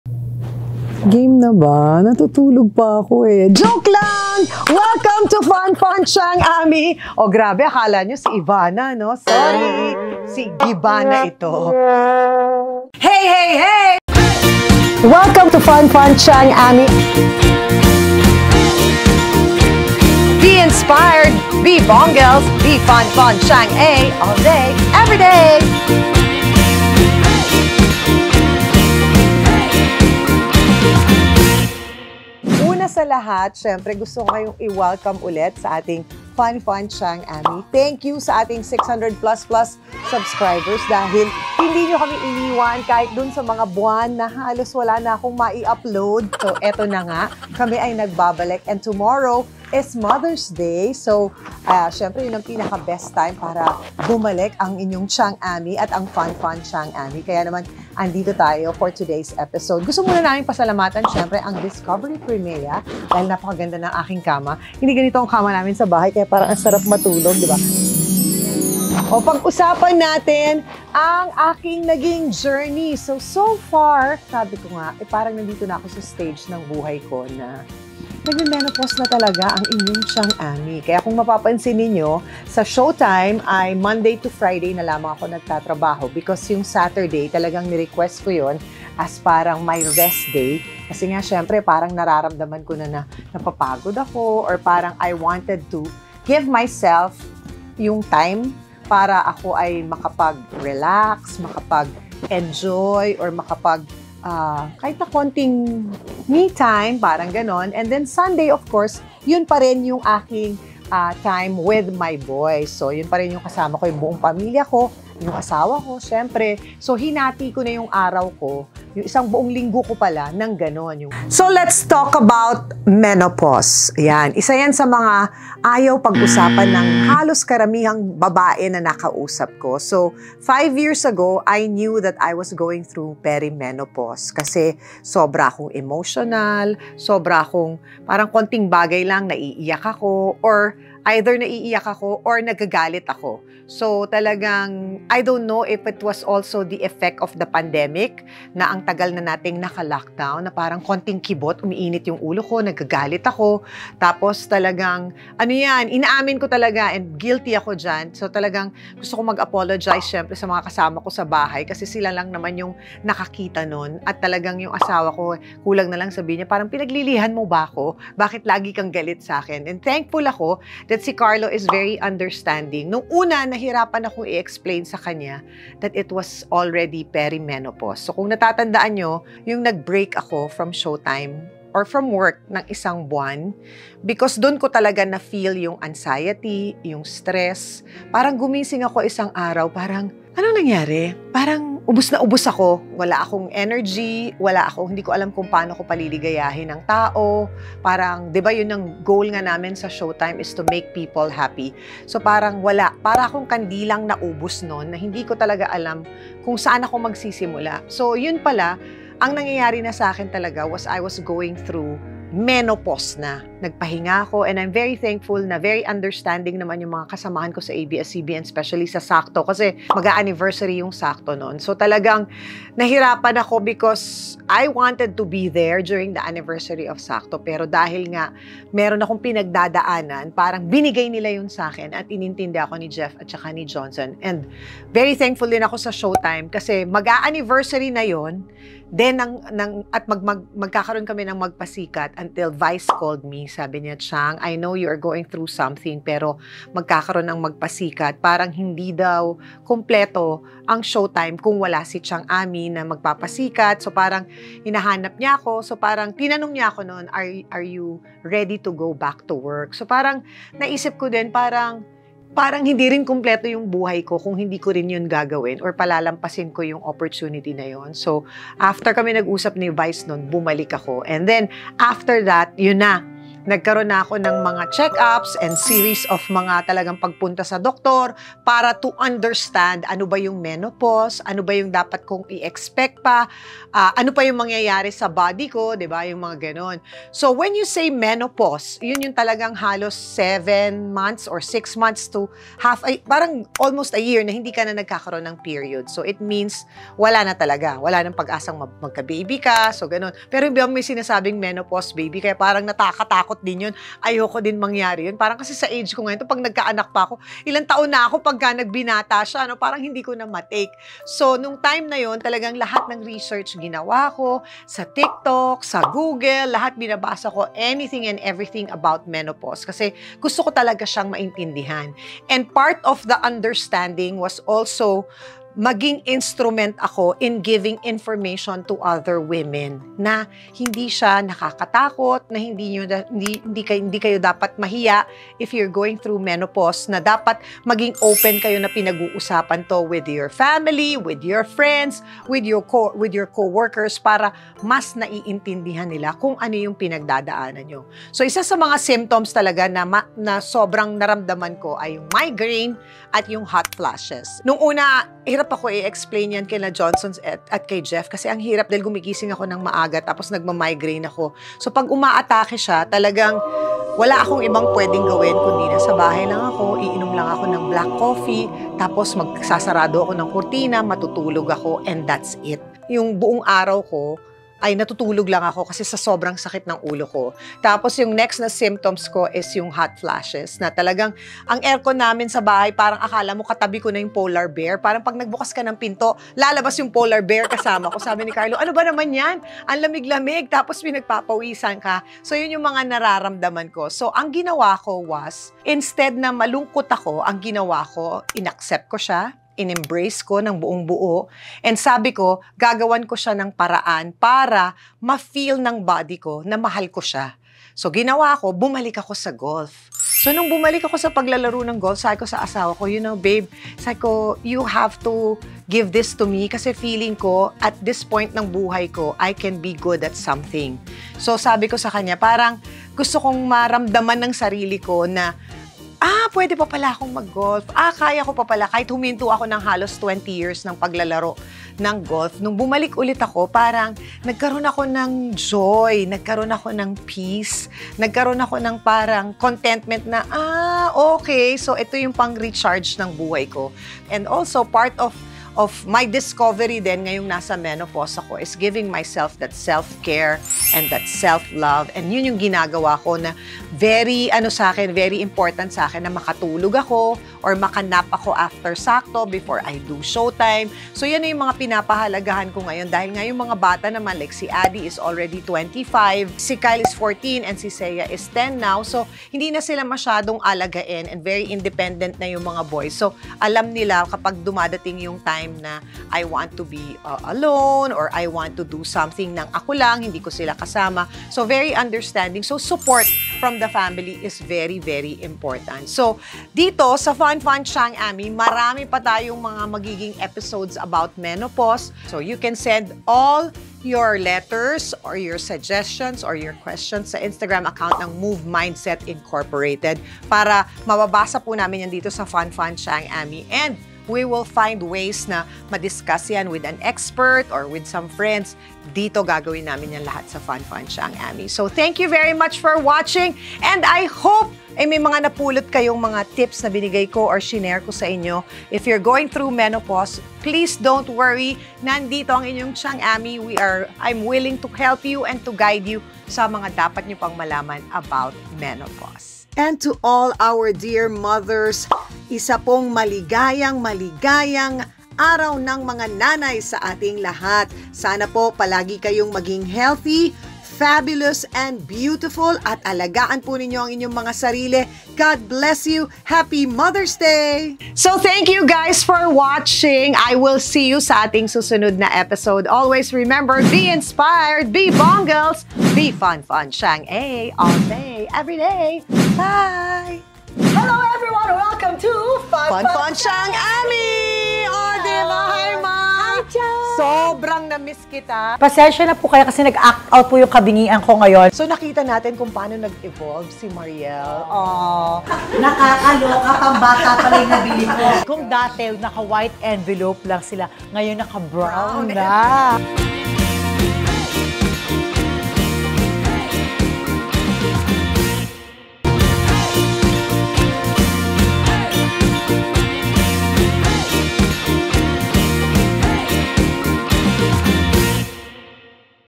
BAKIT MAHIRAP HARAPIN ANG MENOPAUSE? Is This The End? | Fun Fun Tyang Amy Vlog 118
Bakit nga ba nahihiya ang iba na harapin o tanggapin ang menopause? Few years ago, I started my menopause journey and it wasn’t easy. Buti na lang my family, friends and co-workers are very much supportive kaya unti-unti ay naging madali para sa akin na tanggapin ito. Dahil alam kong hindi ako nag-iisa, naisip ko na maging instrumento sa iba pang mga nanay na pinagdadaanan din ito. My channel is a safe space for all the women (and men too!) out there na may mga problema cause Tyang Amy Is Here For You!